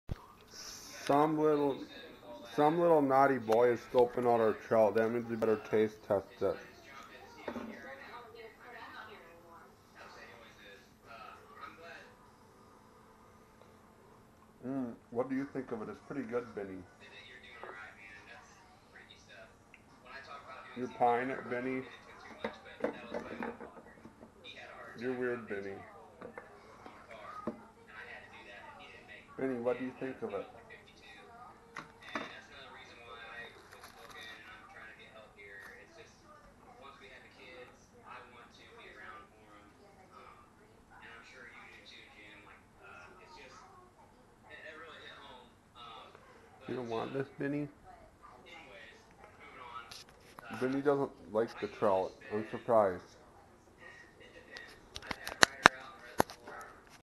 Trying My Dads Catch and Cook
The binnie show being naughty trying to get into musky hans dinner